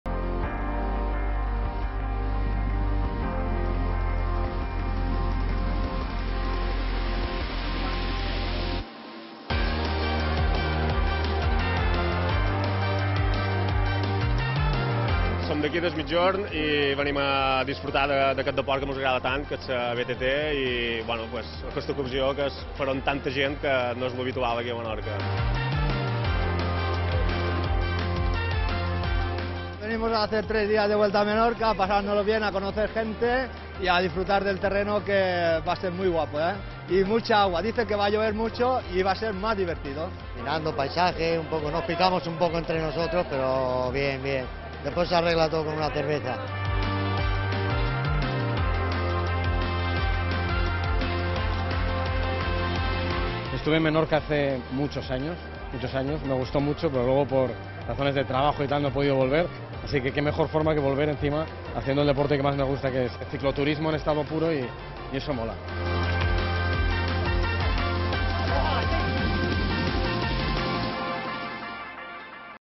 La Volta a Menorca BTT descobreix l'illa a través dels seus camins
Total Esport 16/10/2016
Tota l’actualitat esportiva de les Illes Balears